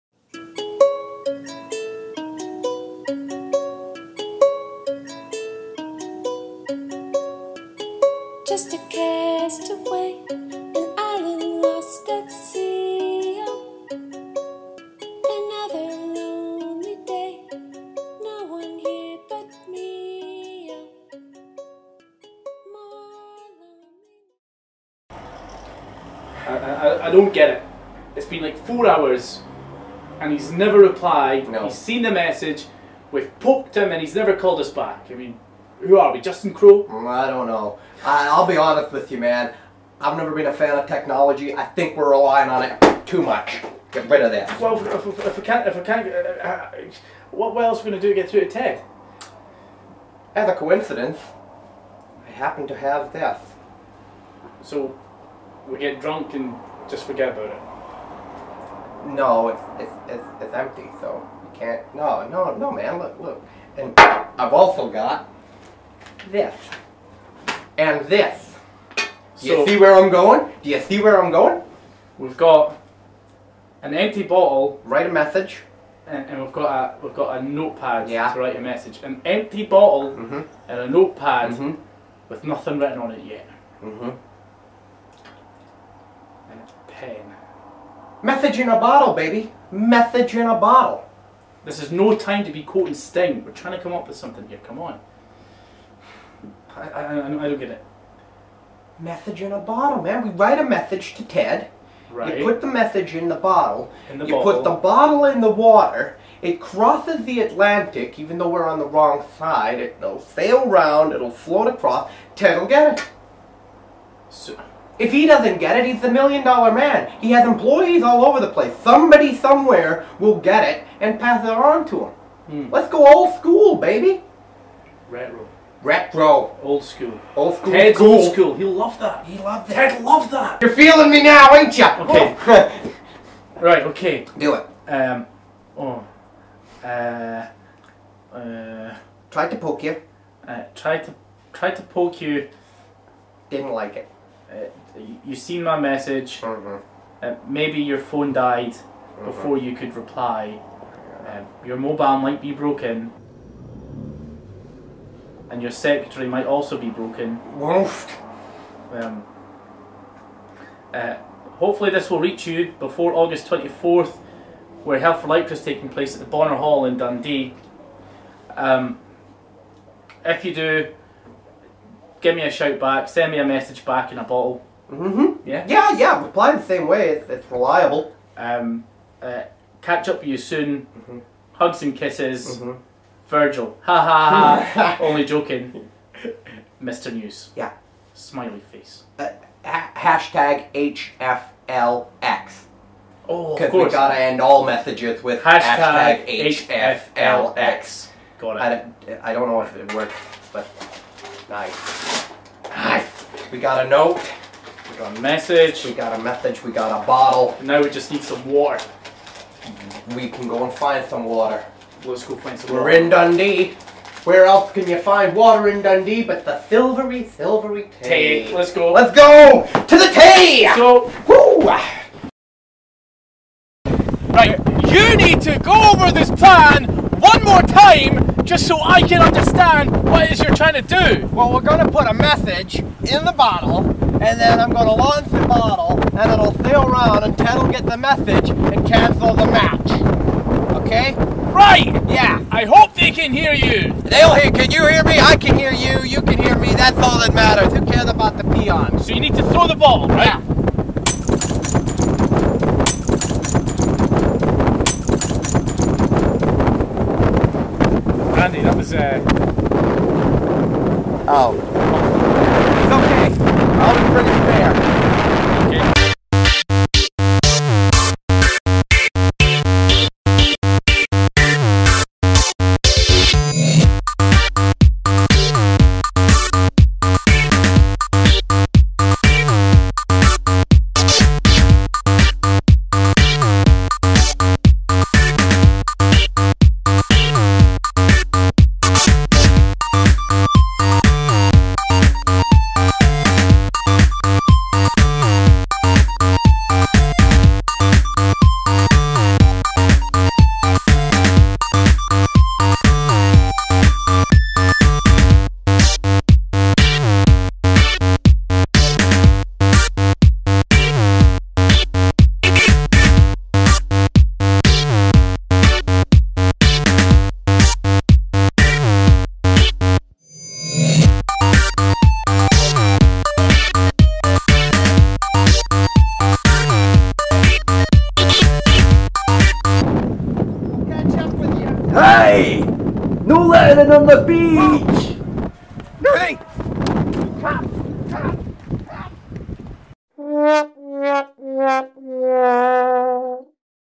SWE Reaching Ted DiBiase - Part 3
Mr News and Randy Valentine are on a frantic mission to get in touch with the Million Dollar man Ted DiBiase regarding Hell For Lycra 10!